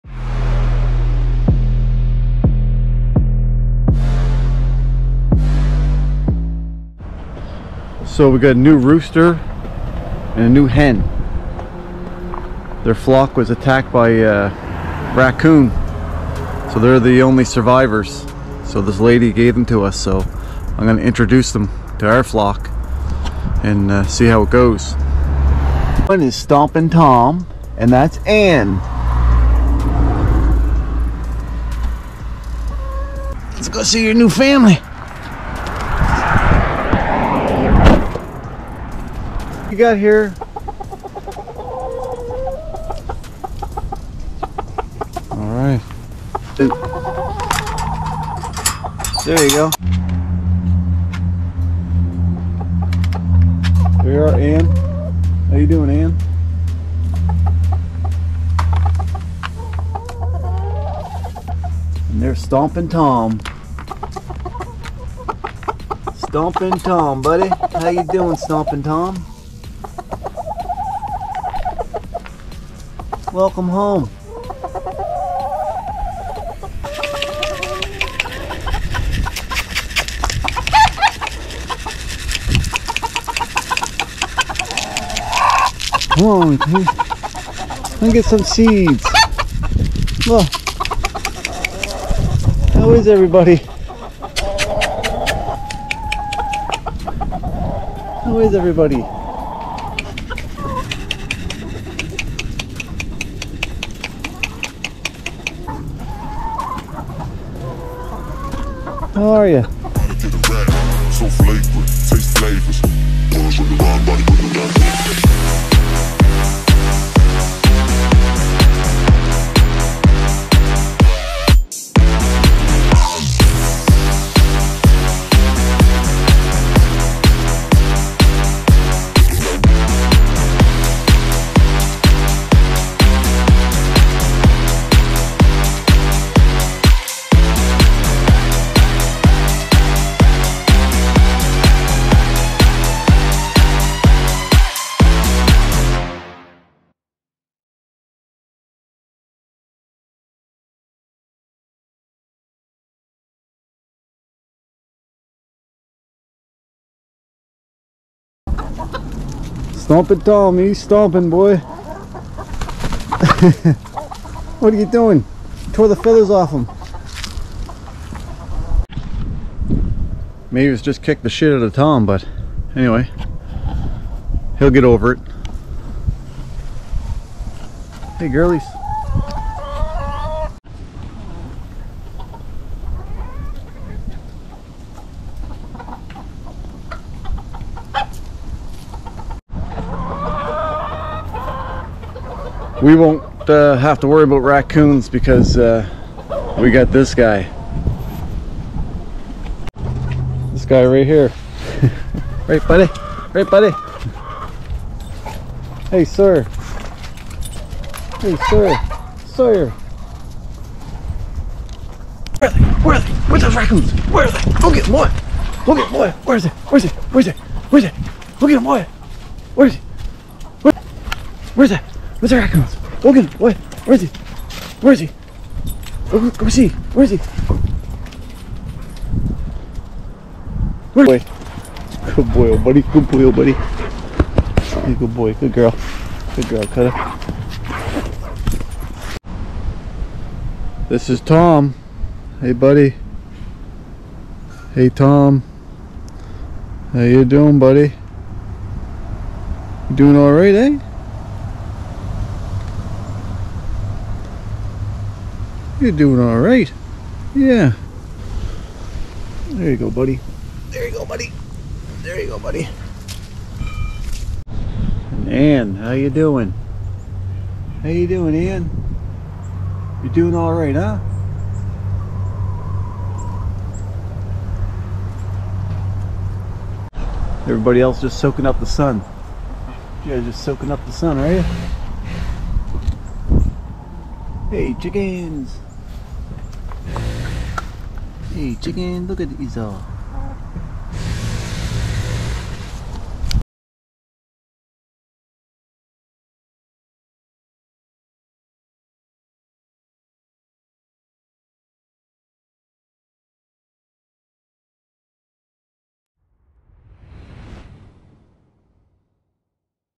Survivors of a raccoon attack
These 2 chickens are the only survivors of a raccoon attack on there flockk